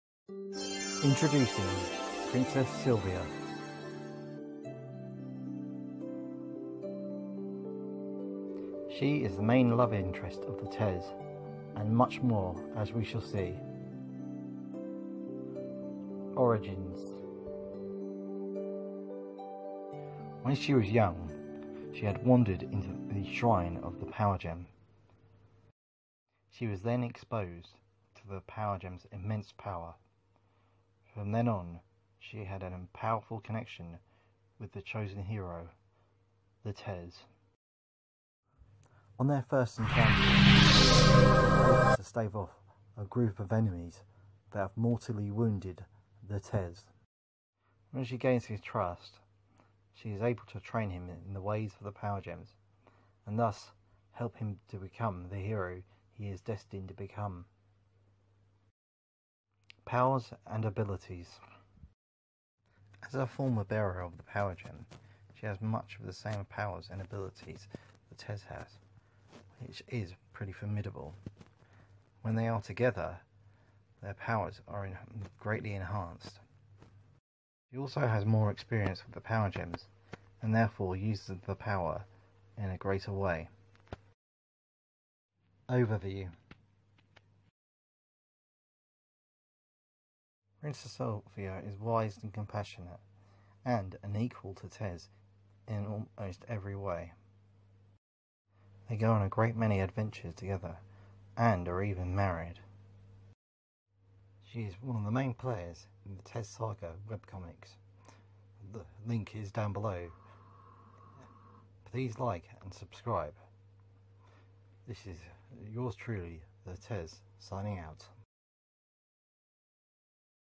Princess Sylvia profile Tez Saga
She is a major player in the Tez saga and a hero in her own right.